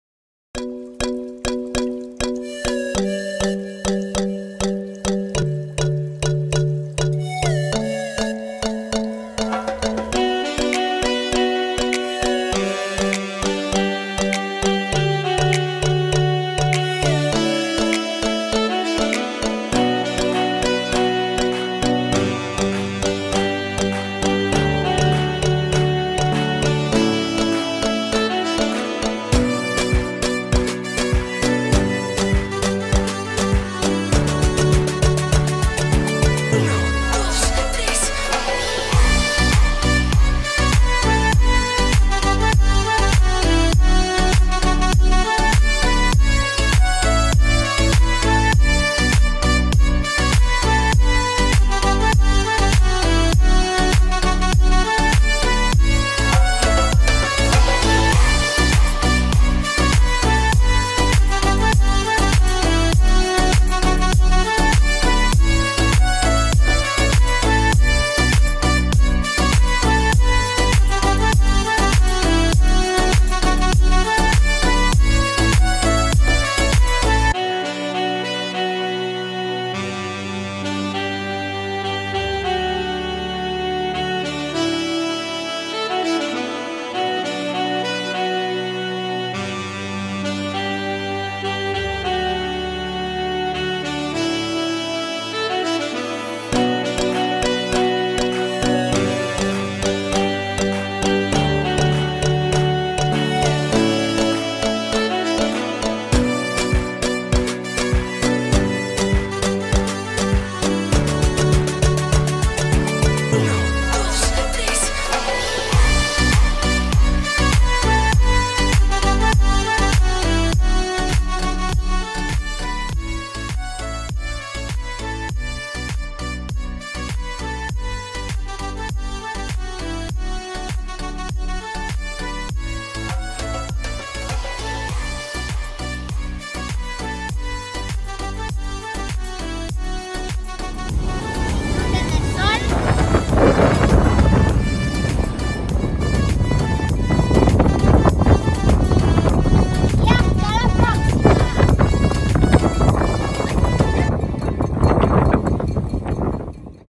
Kite surf | 2020-2021
Este año aprendí a hacer kite y es buenisimo🤣
Disfruta😉